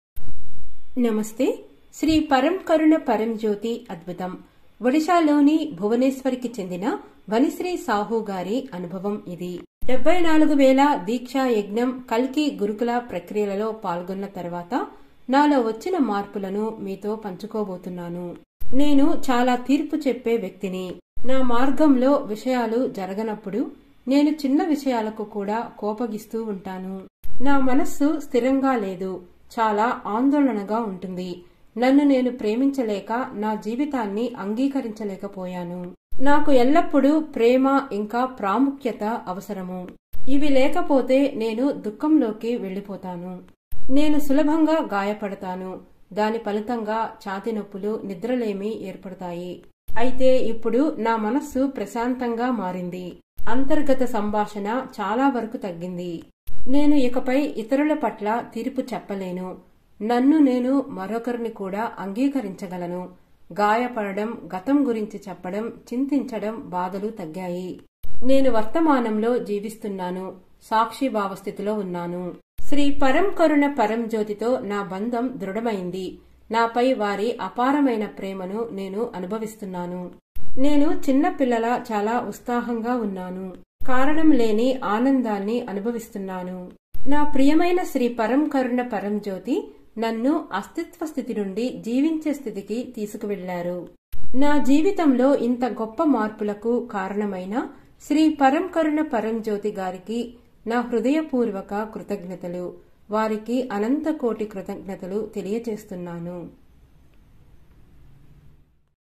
Telugu 25/05. My mind has become calm and the inner dialogue has reduced to a great extent.
I am going to share with you the changes that have happened within me after participating in 74000 Deeksha Yajna and Kalki Gurukul processes. My dear Sri Paramkaruna Paramjyoti have taken me from a state of existing to a state of living. I offer Gratitude from the core of my heart and infinite thanks to Sri Paramkaruna Paramjyoti for such great changes in my life.